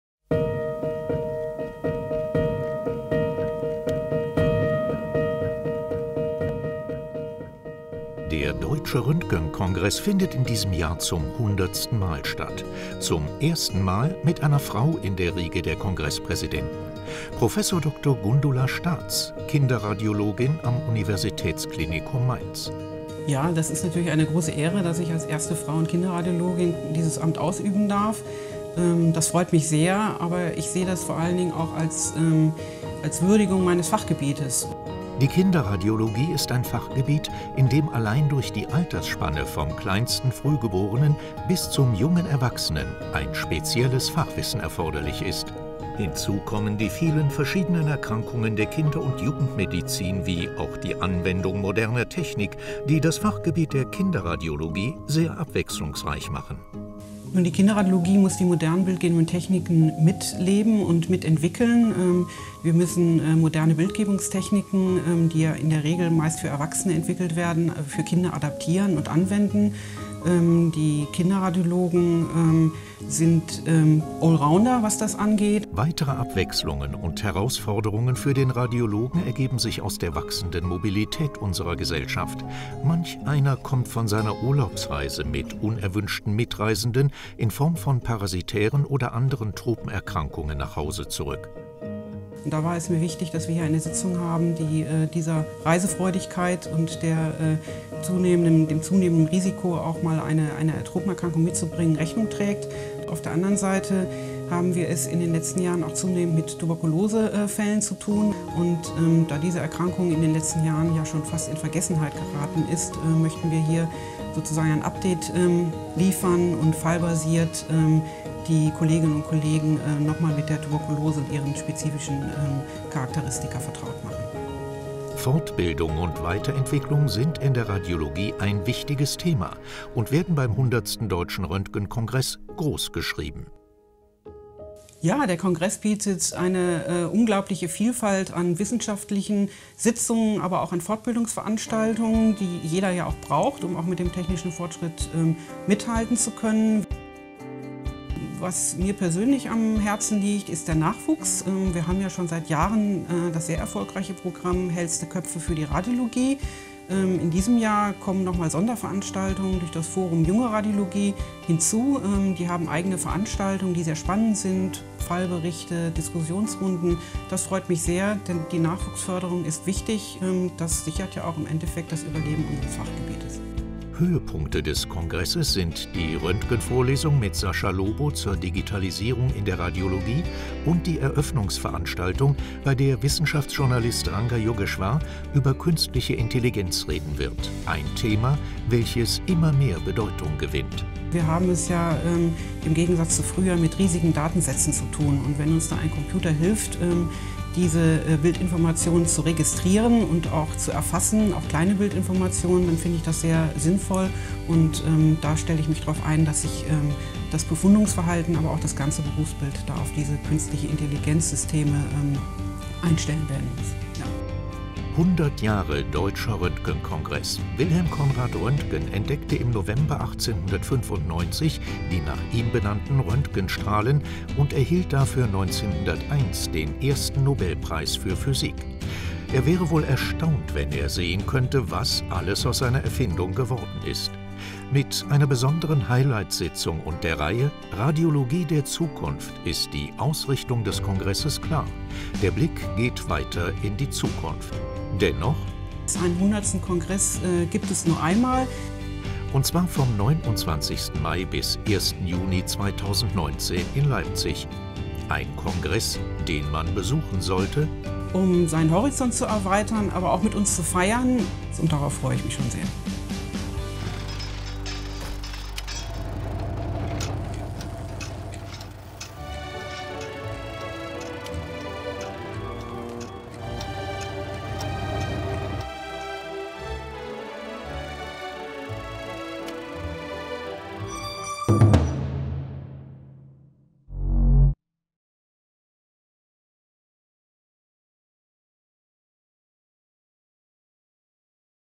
Kurzfilm zum 100. Deutschen Röntgenkongress mit Univ.-Professorin Dr. med. Gundula Staatz
Der 100. Deutsche Röntgenkongress findet vom 29. Mai bis 1. Juni im Congress-Center Leipzig statt. Im Film spricht die Kongresspräsidentin Univ.-Professorin Dr. med. Gundula Staatz über Kinderradiologie und aktuelle Herausforderungen.